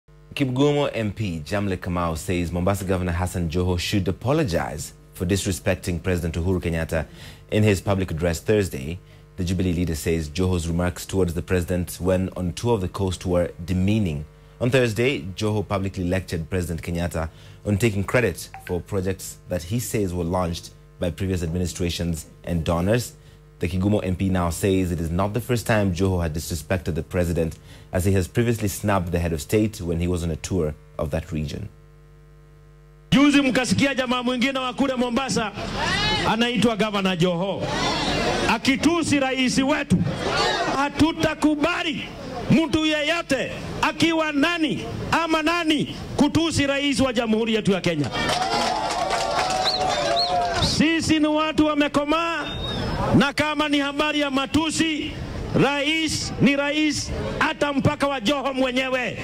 Kigumo MP Jamleck Kamau wants Governor Joho to apologise to the president
Kigumo MP Jamleck Kamau says Mombasa Governor Hassan Joho should apologise for disrespecting President Uhuru Kenyatta in his public address. The Jubilee leader says Joho's remarks towards the president, when on a tour of the Coast, were demeaning. Joho publicly lectured President Kenyatta on taking credit for projects that he says were launched by previous administrations and donors. The Kigumo MP says its not the first time Joho has disrespected the president, as he has previously snubbed the head of state when he was on a tour of the region.